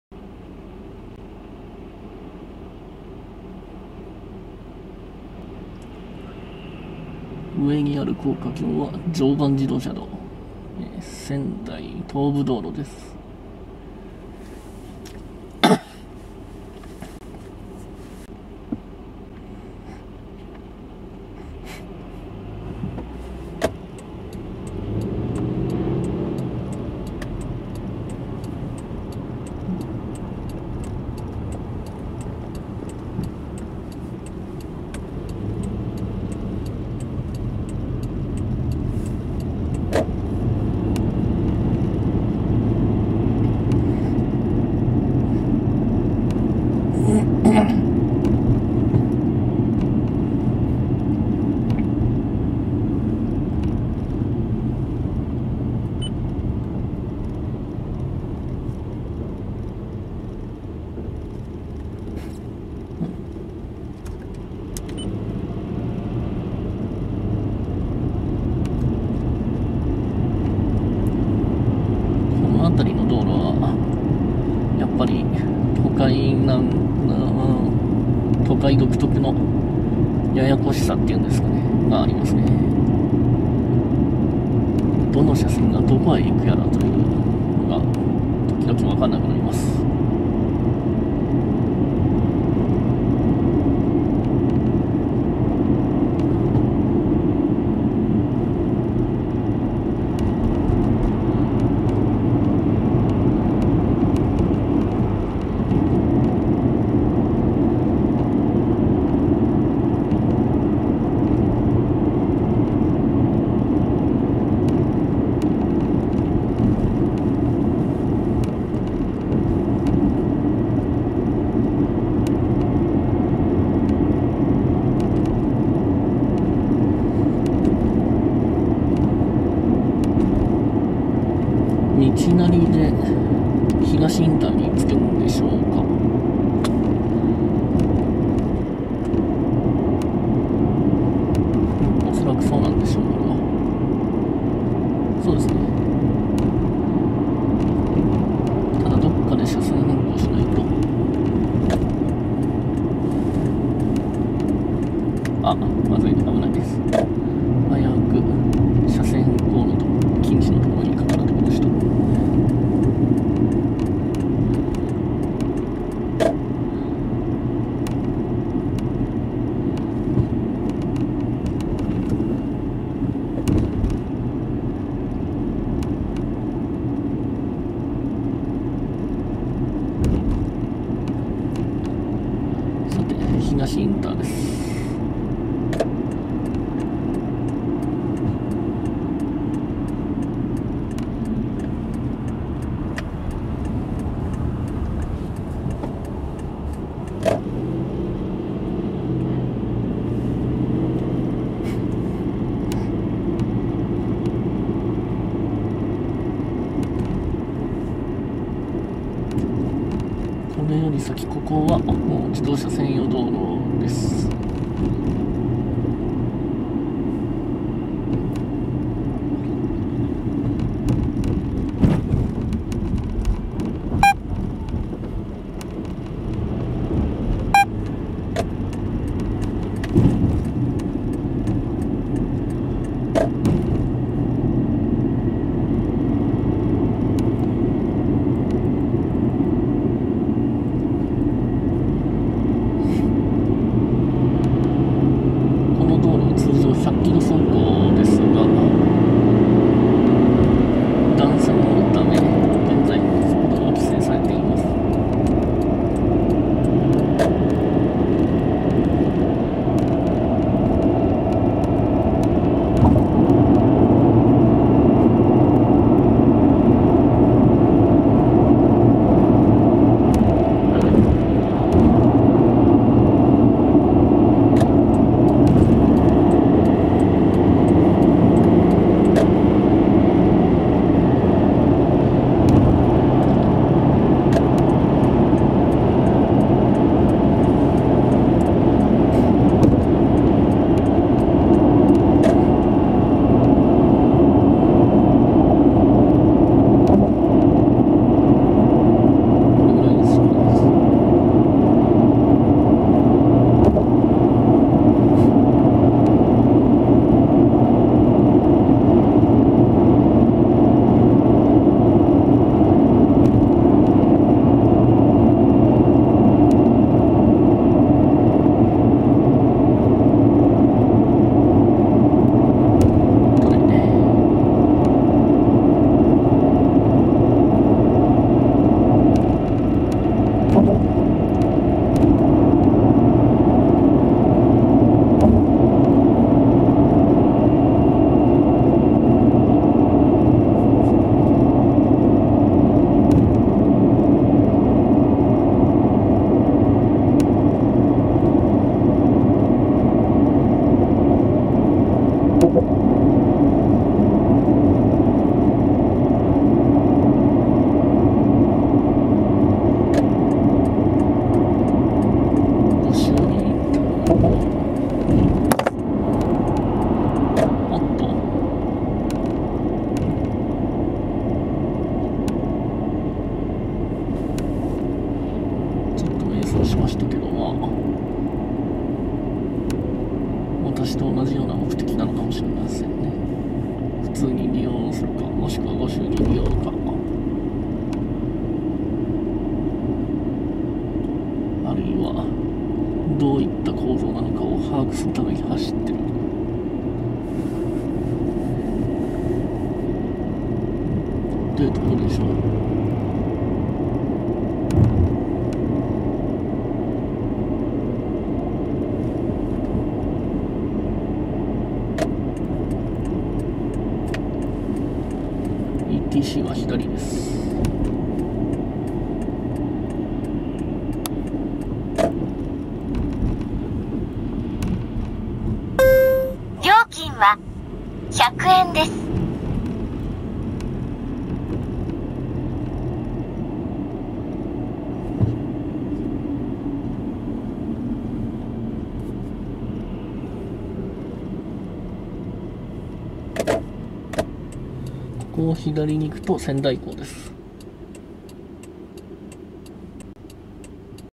20121202 常磐道 下り 仙台東IC→仙台港IC(Dランプ)
常磐自動車道は東京都練馬区から水戸市、いわき市、原町区などを経て宮城県仙台市に至る高速自動車国道です。

仙台東部道路は常磐道の役割をこなし、仙台環状道路の一部を形成して仙台圏の交通網を支える重要な道路です。

2012年12月1日、本道路に仙台港ICという新たなインターチェンジが開設されました。仙台港ICは三郷JCTから323.4km付近に、亘理ICから23.0km付近にあります。本動画は仙台港ICの様子を撮影するとともに、Dランプを通過する動画です。

Dランプは下り本線から、IC出口へ向かうランプです。